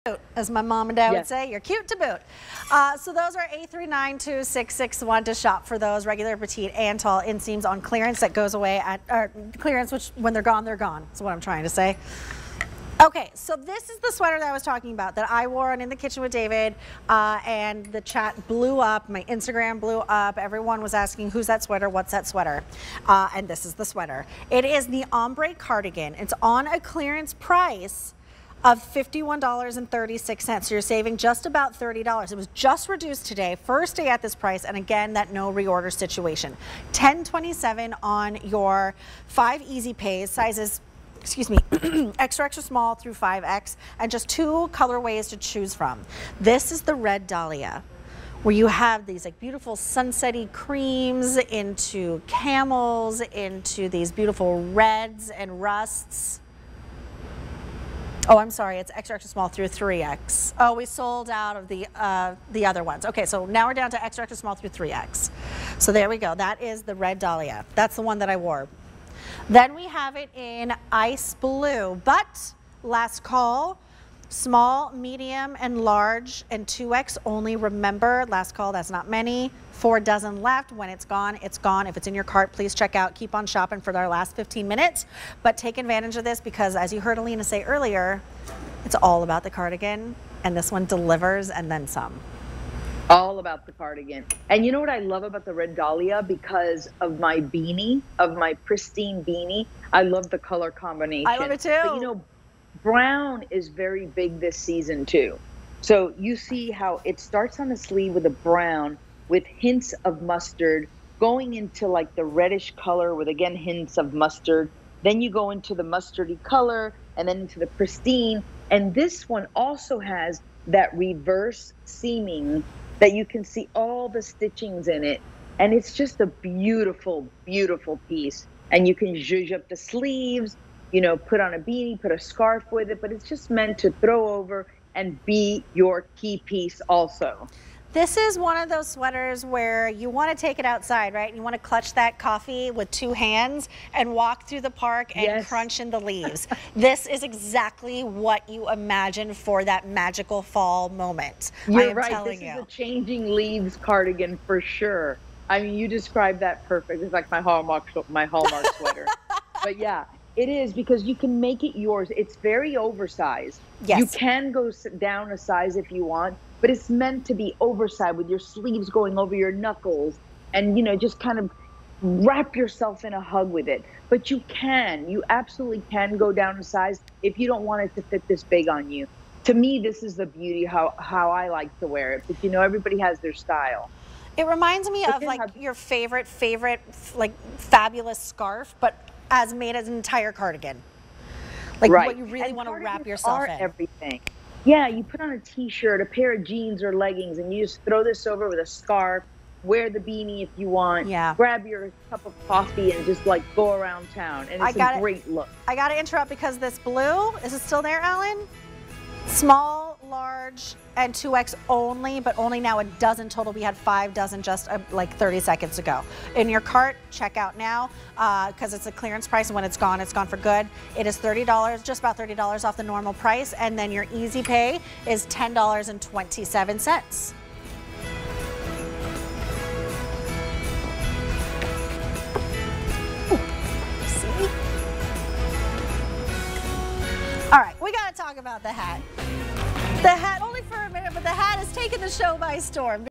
Peace Love World Ombre Cardigan on QVC
Peace Love World Ombre Cardigan

Okay, we are loving this cozy cardigan for your coffee dates to the park or comfy movie nights in. Wrap yourself in warmth and adorn yourself in adorable ombre print. From Peace Love World.